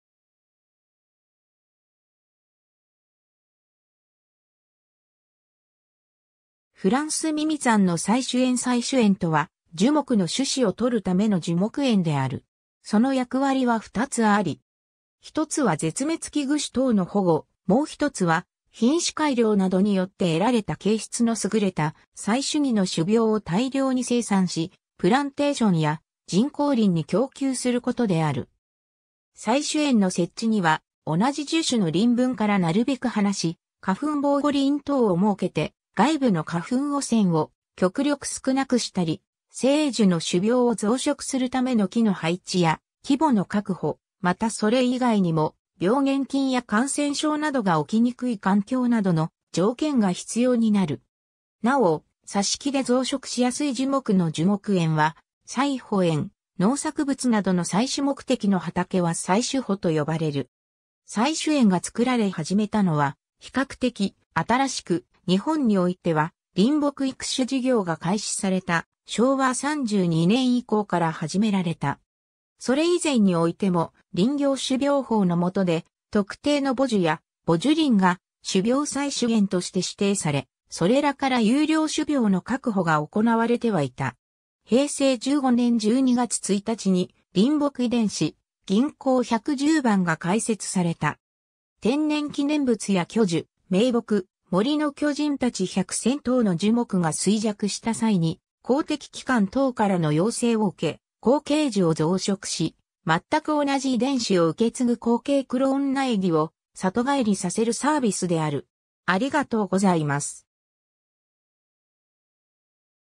採種園
フランスミミザンの採種園 採種園（英語：seed orchard）とは、樹木の種子を採るための樹木園である。
その役割は2つあり。
一つは絶滅危惧種等の保護、もう一つは品種改良などによって得られた形質のすぐれた採種木（精英樹、エリートツリー、英語：elite tree）の種苗を大量に生産し、プランテーションや人工林に供給する事である。
 採種園の設置には、同じ樹種の林分からなるべく離し、花粉防護林等を設けて外部の花粉汚染を極力少なくしたり、精英樹の種苗を増殖するための樹の配置や規模の確保、またそれ以外にも病原菌や感染症などが起きにくい環境などの条件が必要になる。
 なお、挿木で増殖しやすい樹木の樹木園は採穂園、農作物などの採種目的の畑は採種圃（英：seed farm）と呼ばれる。
 採種園が造られ始めたのは比較的新しく、日本においては林木育種事業が開始された昭和32年以降から始められた。
それ以前においても、林業種苗法の下で特定の母樹や母樹林が種苗採取源として指定され、それらから優良種苗の確保が行われてはいた。
 平成15年12月１日に「林木遺伝子銀行110番」が開設された。
天然記念物や巨樹、名木、森の巨人たち百選等の樹木が衰弱した際に、公的機関等からの要請を受け、後継樹を増殖し、全く同じ遺伝子を受け継ぐ後継クローン苗木を里帰りさせるサービスである。